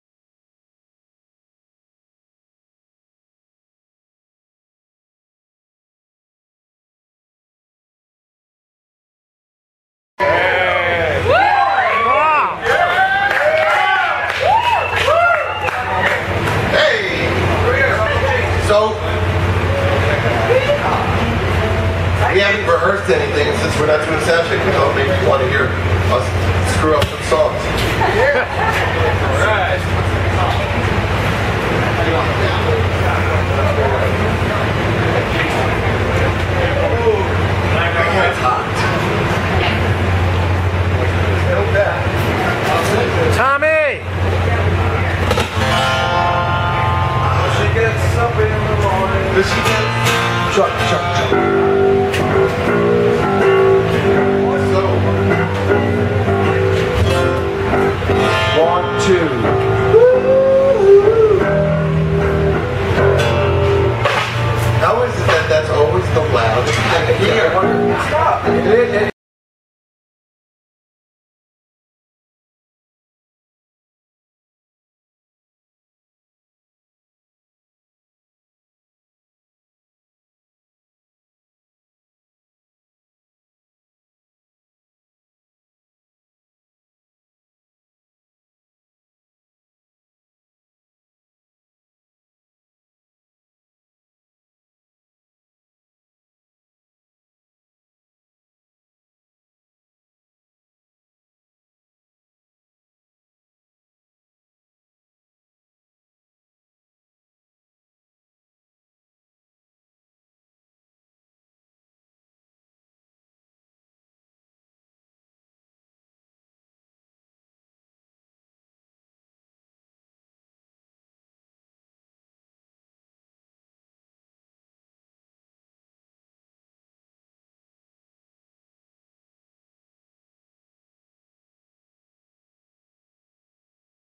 KISS WPB 07272012
Short clip of KISS backstage during acoustic session and some photos from the first row. More to follow...